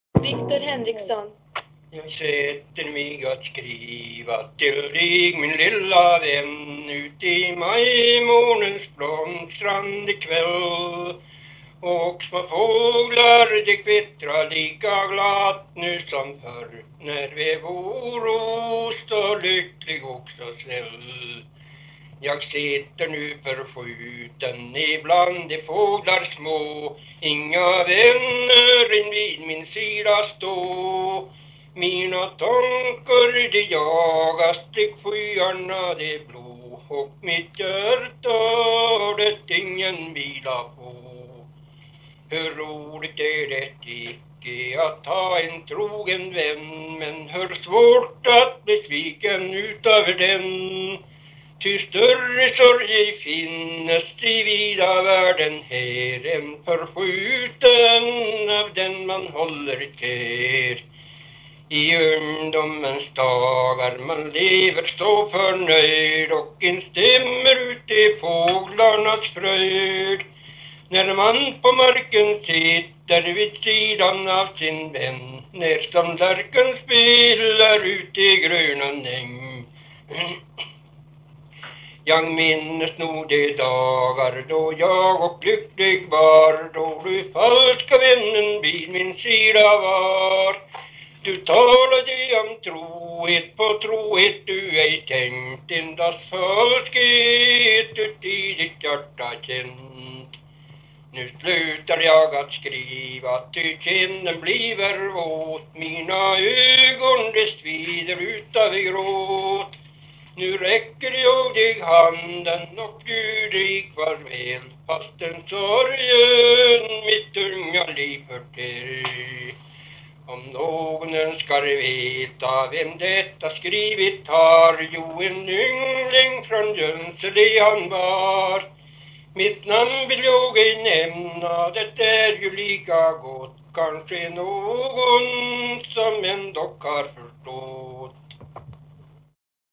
Sång av Viktor Henriksson, Junsele (1883-1971)
Min mormors far, datum för inspelning okänt, men inspelningen fanns på band tillhörande en gammal rullbandare som gissningsvis är från 50-talet. Det kan vara inspelat i slutet av 50-talet. Viktor har skrivit texten själv, melodin är ett skillingtryck. Förlagan som Viktor skrivit om och moderniserat verkar vara den här